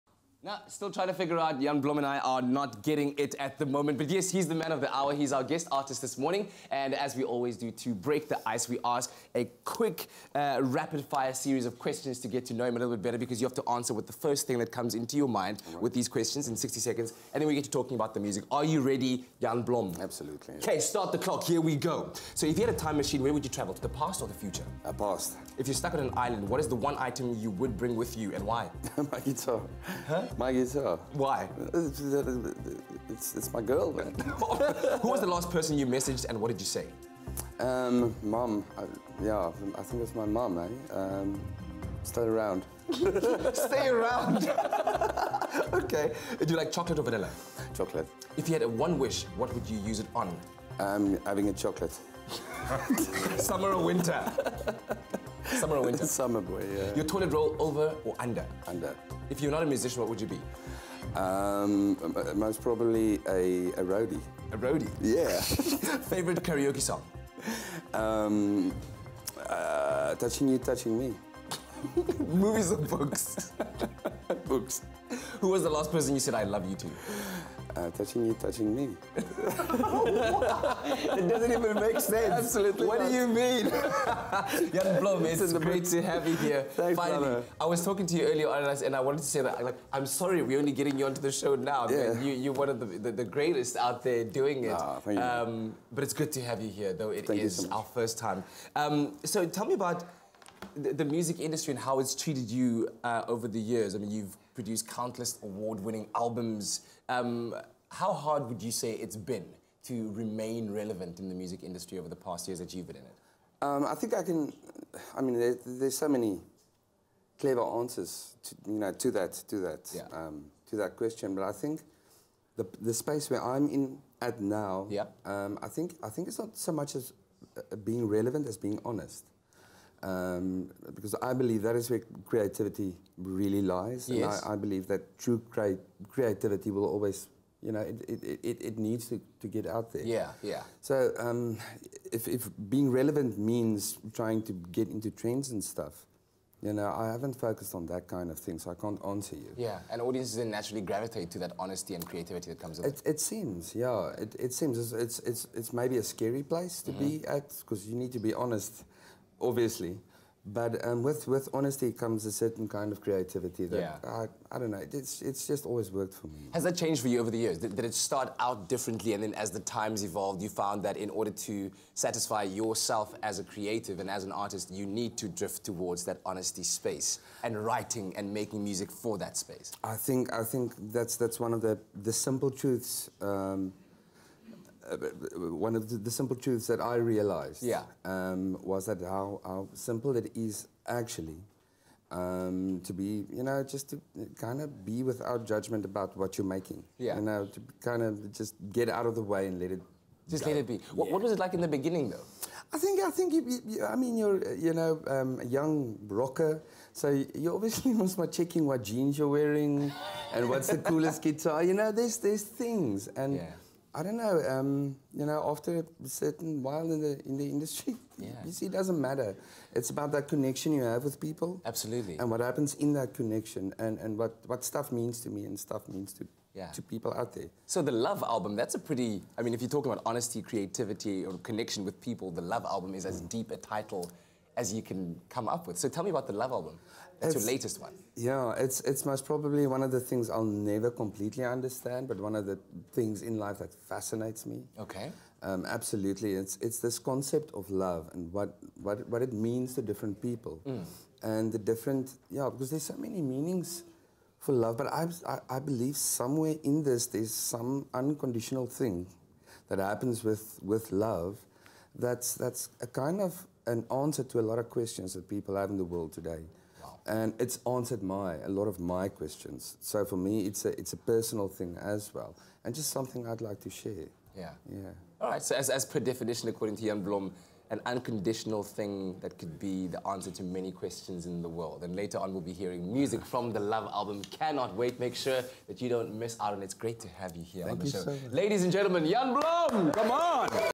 Getting to know Jan Blohm
We get to know Jan Blohm a little better with a round of quick-fire questions.